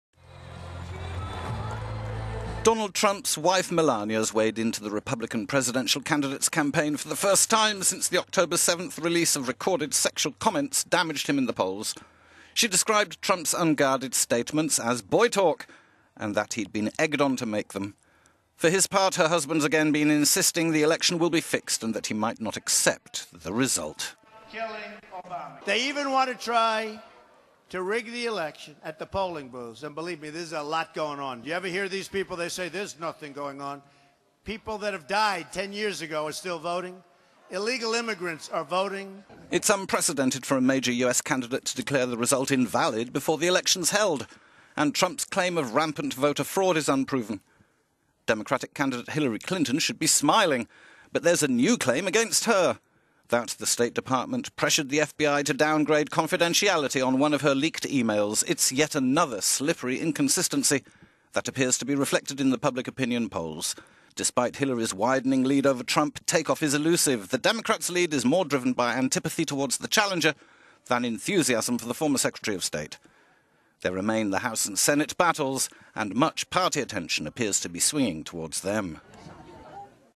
Melania defends husband's "boy talk", Trump repeats "rigged election" claim
Donald Trump's wife Melania has weighed into the Republican presidential candidate's campaign for the first time since the October 7 release of recorded sexual comments damaged him in the polls.

She described Trump's 2005 unguarded statements as "boy talk" and that he had been "egged on" to make them by television host Billy Bush.

For his part her husband has again been insisted the election will be fixed and that he might not accept the result, insisting "people dead for 10 years"